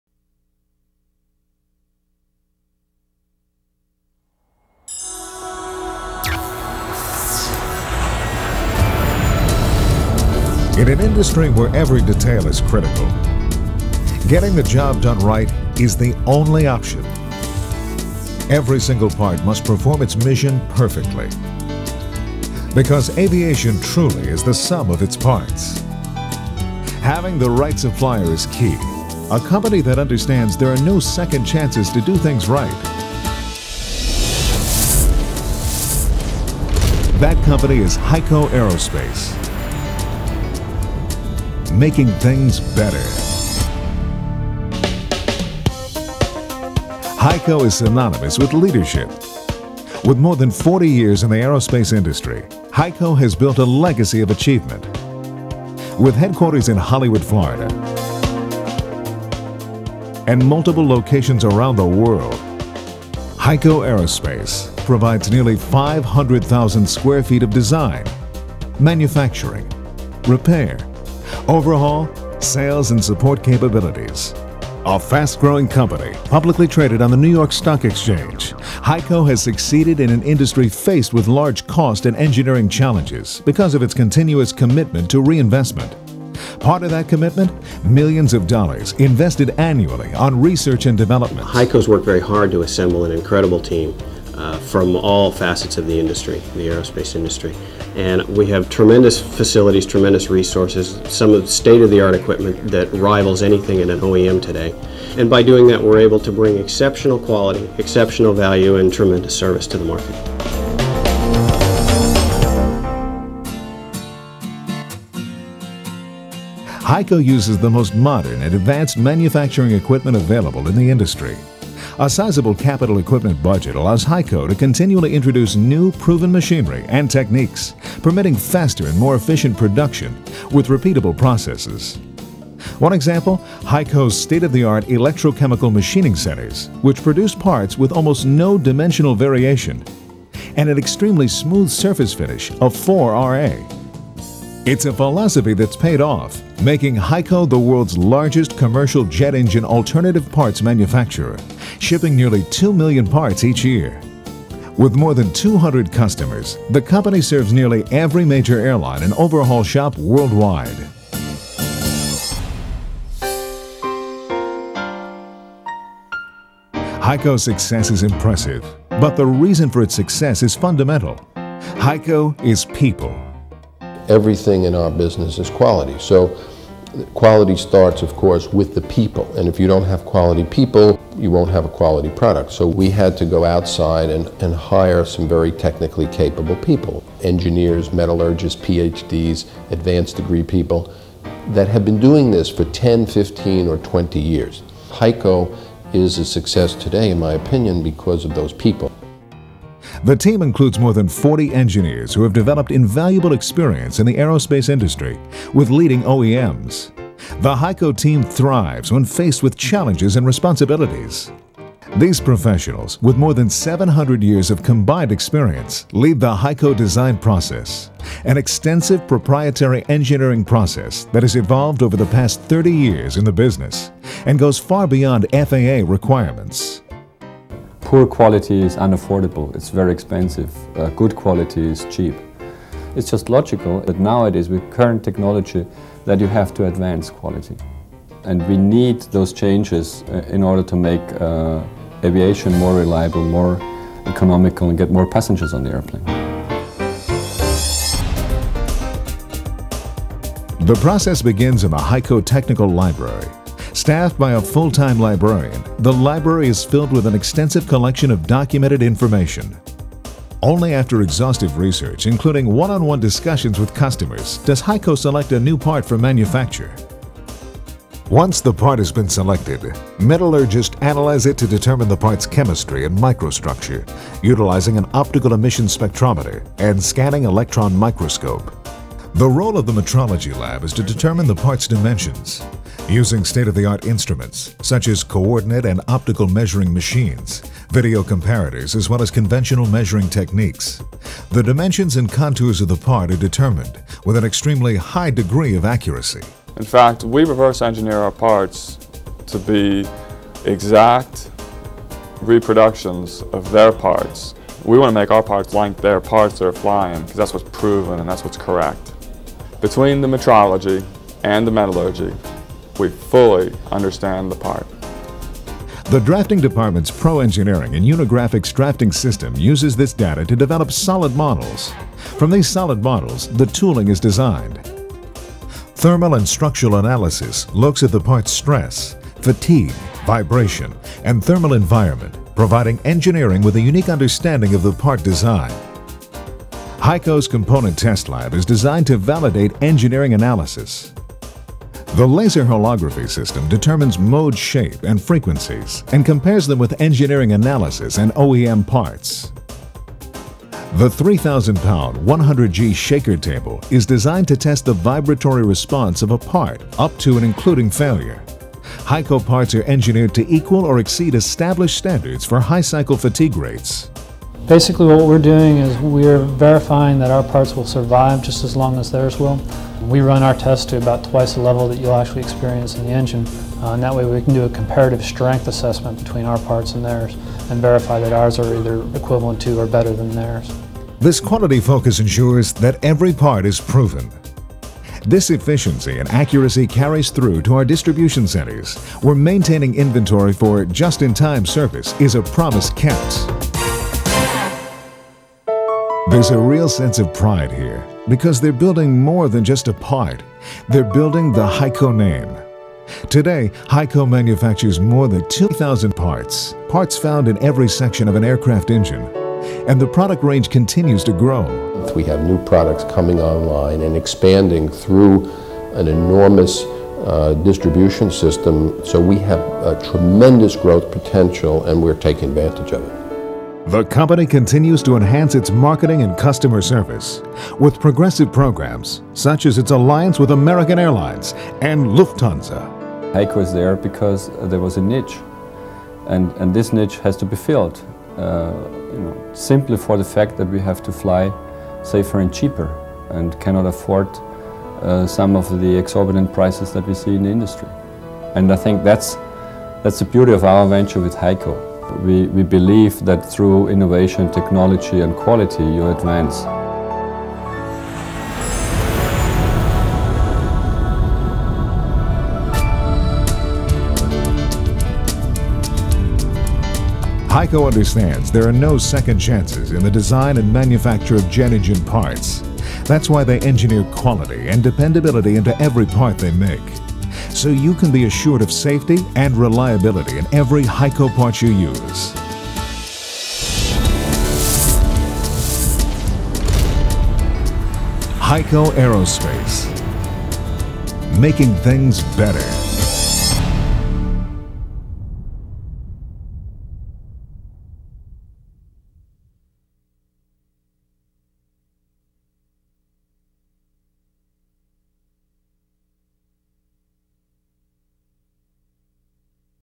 Heico Corporation - "Making things better"
Winner Vision Award.  HEICO Corporation designs, manufactures and sells aerospace, defense and electronics related products and services throughout the world. The company is headquartered in Hollywood, Florida and its  customer base is primarily the commercial aviation, defense, space, medical, telecommunication and electronic industries..

This is their company video promotion produced in 2001.

Winner Vision Award.

A Kyle Farley media production.